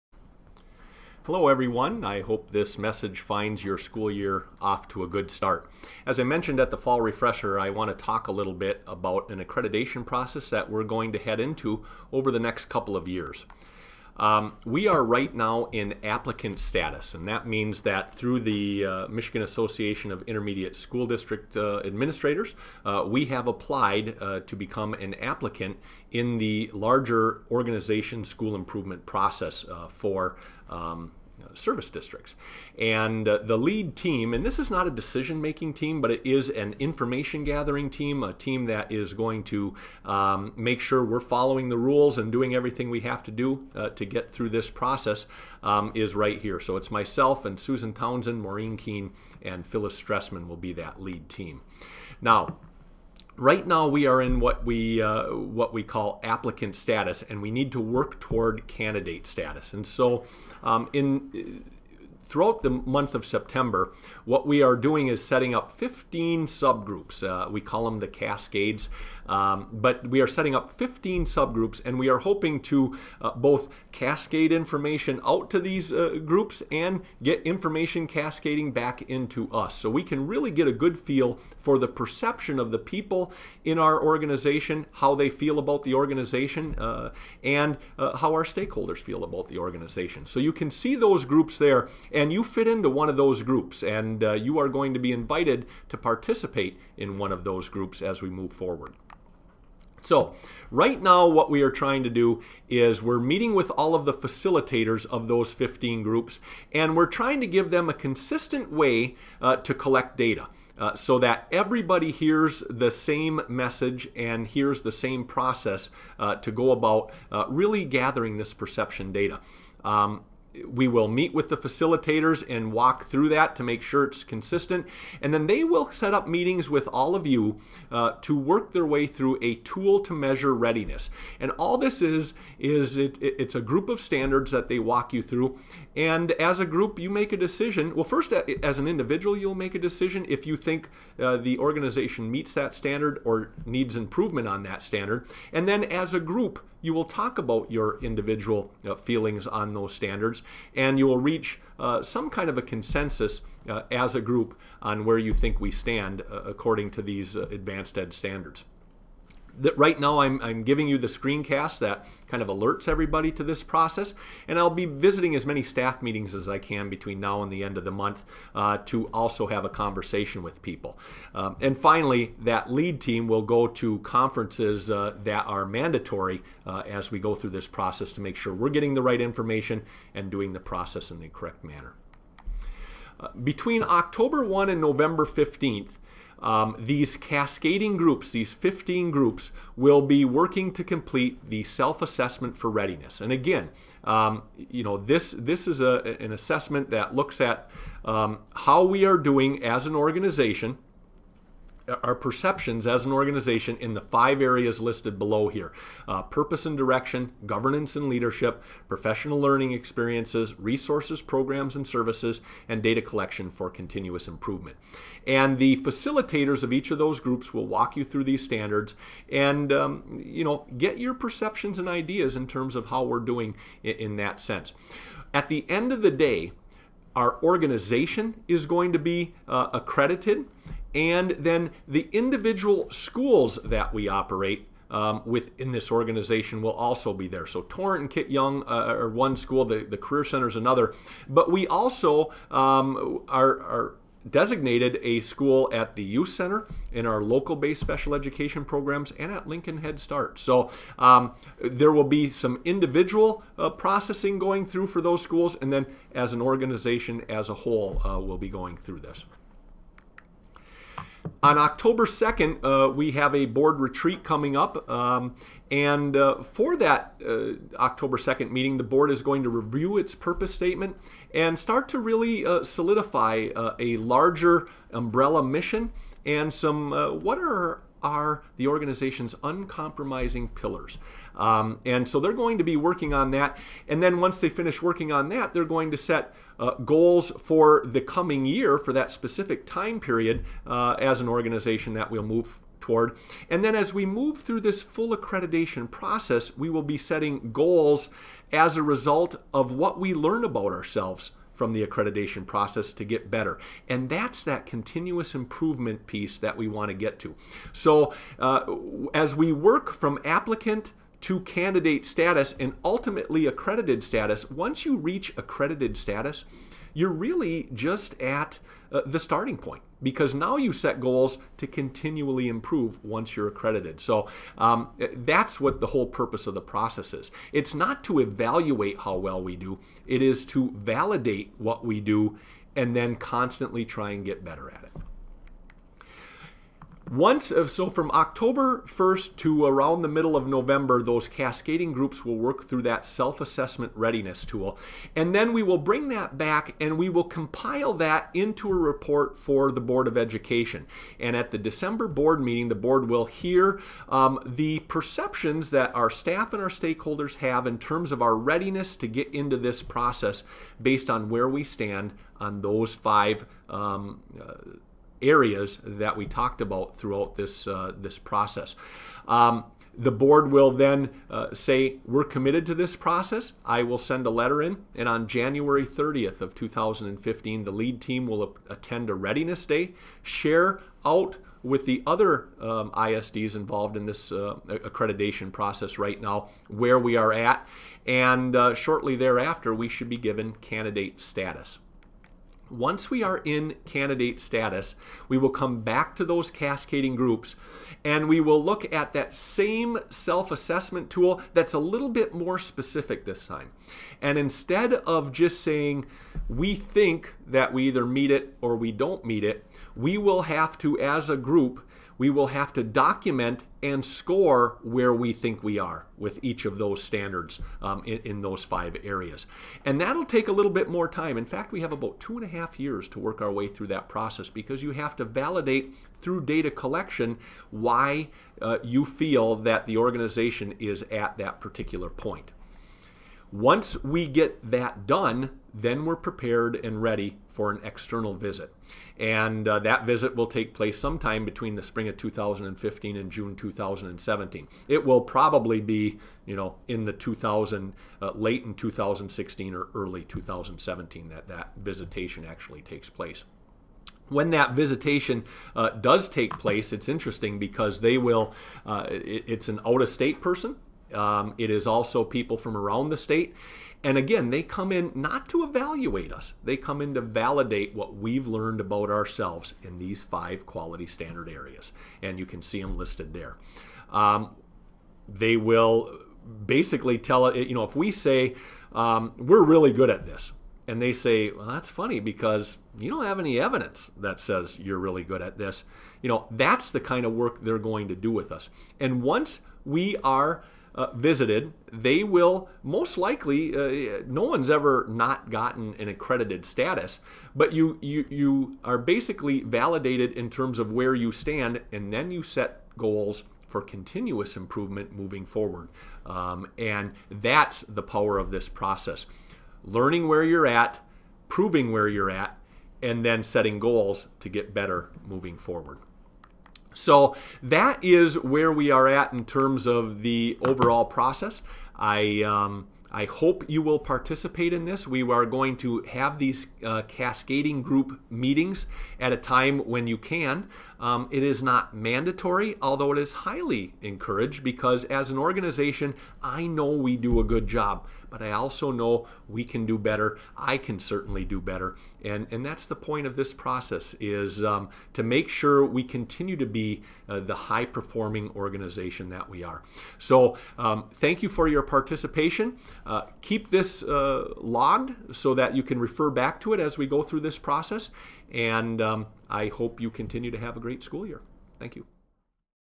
JCISD Accreditation Process 9/2014
JCISD Superintendent, Kevin Oxley, explains the upcoming accreditation process.